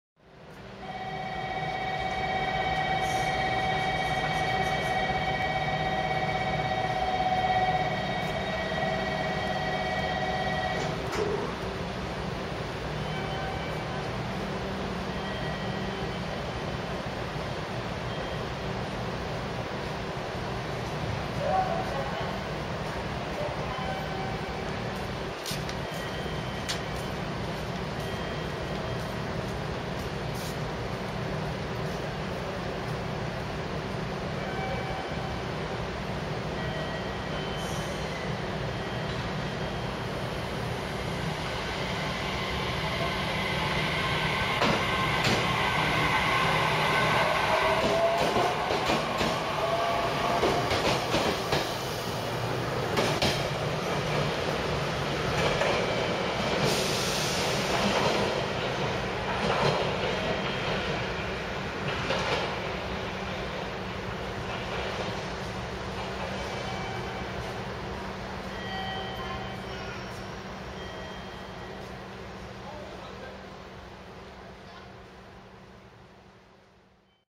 日豊本線817系普通都城行き 鹿児島中央駅発車
鹿児島中央駅4番線を発車する日豊本線普通都城行。817系2両編成。
当駅の所属路線は鹿児島本線で日豊本線は隣の鹿児島駅が終点。しかし、日豊本線の列車は全列車当駅まで直通する。

＝2013/09/13撮影（Panasonic HX-DC2）＝